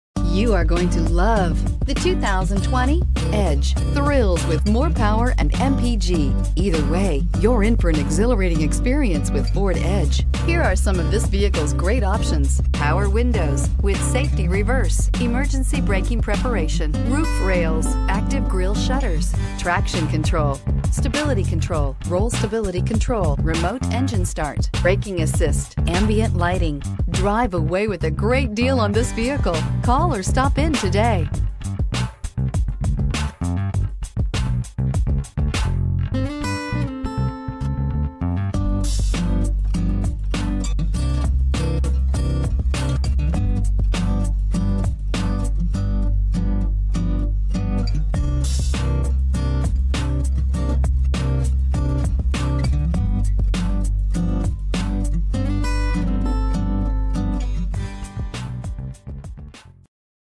2020 Ford Edge New Bern NC T10853
2020 Ford Edge Titanium

Cella Ford
3210 Doctor M.L.K. Jr Blvd

* GREAT DEAL AT $42,705 *   * 2020 ** Ford * * Edge * * Titanium *  Buckle up for the ride of a lifetime! This 2020 Ford Edge Titanium includes a top-notch backup sensor, push button start, remote starter, backup camera, blind spot sensors, parking assistance, Bluetooth, braking assist, dual climate control, and hill start assist.  Rocking a suave dark exterior and a medium soft ceramic interior, this car is a sight to see from the inside out.  Want a crossover you can rely on? This one has a crash test rating of 5 out of 5 stars!  Want to learn more? Call today for more information.